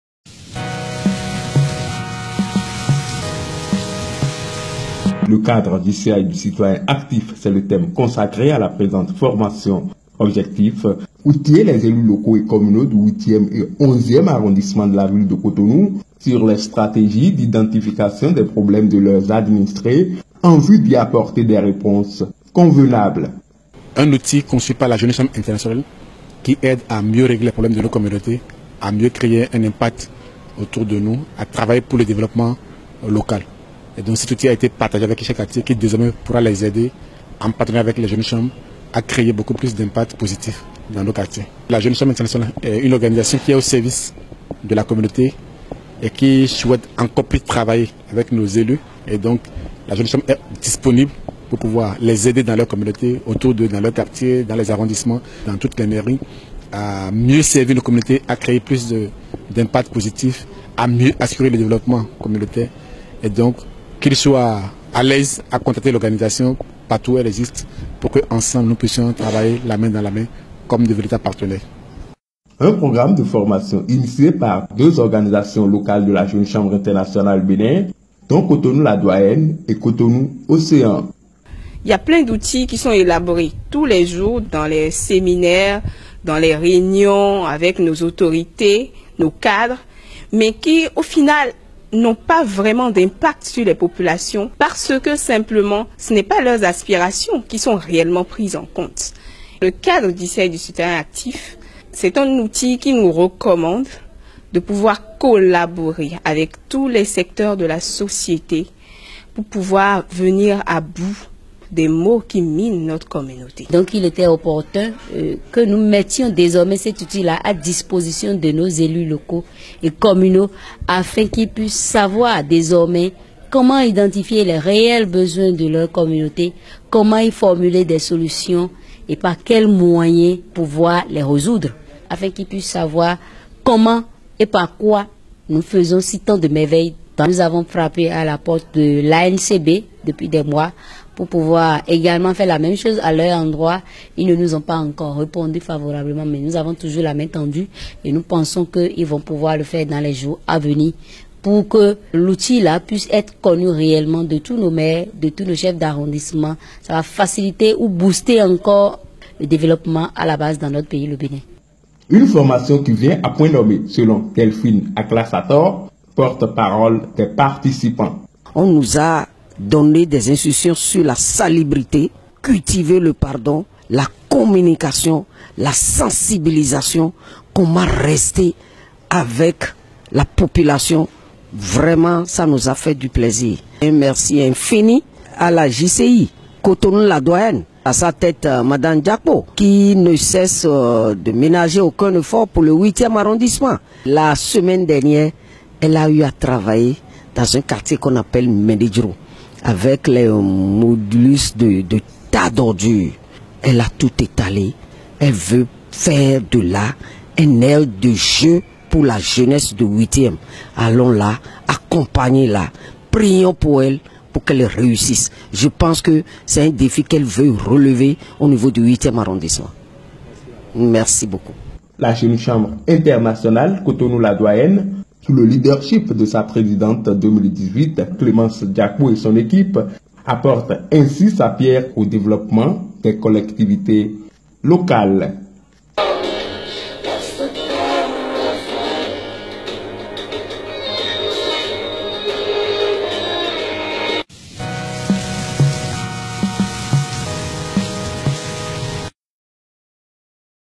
La JCI Cotonou La Doyenne apporte sa touche au développement des collectivités locales.
Bénin : La JCI Cotonou La Doyenne apporte sa touche au développement des collectivités locales du 8ème et 11 ème arrondissement de la ville de Cotonou. Les élus locaux ont reçu une formation sur le cadre JCI du Citoyen Actif. Un outil qui leur permettra d'identifier les réels besoins de leurs mandants en vue d'y apporter des solutions adéquates. Roosvelt NOUMEDON et Angelos AZAGBA pour
Awissi Web tv..